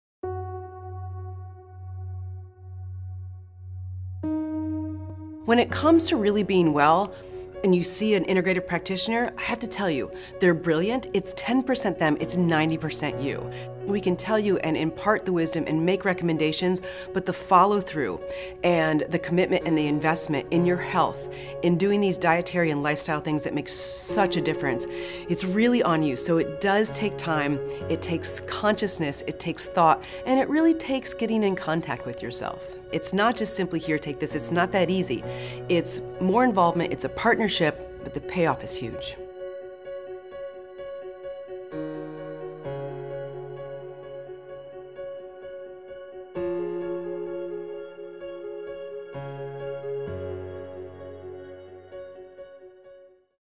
Holly Lucille - True healing takes commitment from the patient - Integrative Wisdom
Dr. Holly Lucille doesn’t mince words when explaining the importance of investing in your own health. Integrative practitioner provide more than advice, she explains. It’s a partnership that, in time, yields huge results.

The Interview
When it comes to really being well and seeing an integrative practitioner, they're brilliant, but it's 10% them, and 90% you. We can tell you and impart the wisdom and make recommendations, but the follow-through and the commitment and the investment in your health in doing these dietary and lifestyle things that make such a difference, it's really on you. It does take time, it takes consciousness, it takes thought and it really takes getting in contact with yourself. It's not just simply, "Here, take this." It's not that easy. It's more involvement, it's a partnership, but the payoff is huge.